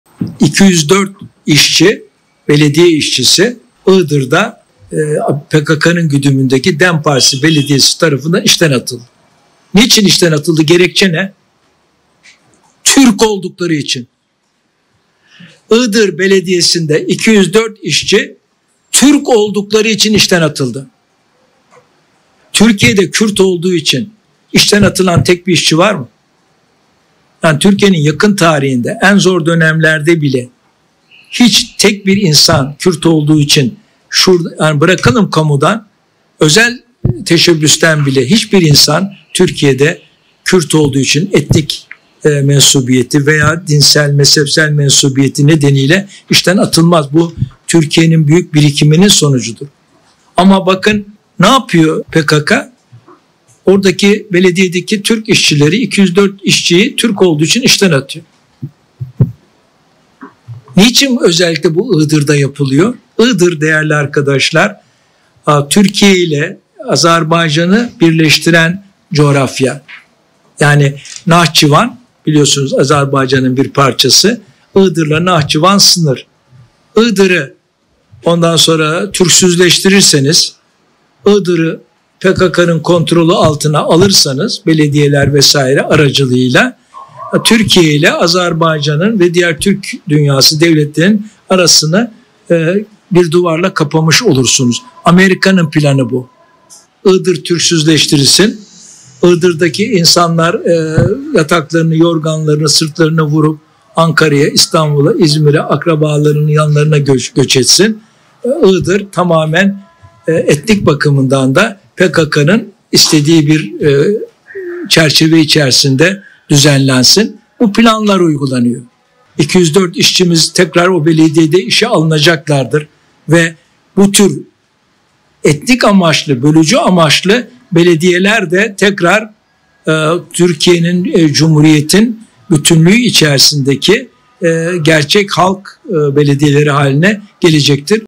Iğdır'da 204 işçi Türk oldukları için işten atıldılar | Vatan Partisi Genel Başkanı Doğu Perinçek
UÇ-724